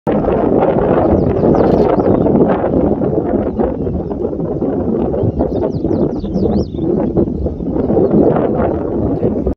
River terns seen preening at Bhigwan grasslands, Maharashtra, India
Species-River tern
Location-Bhigwan, Maharashtra, India
Date-July 2022
Camera-Samsung Galaxy Note S20 Ultra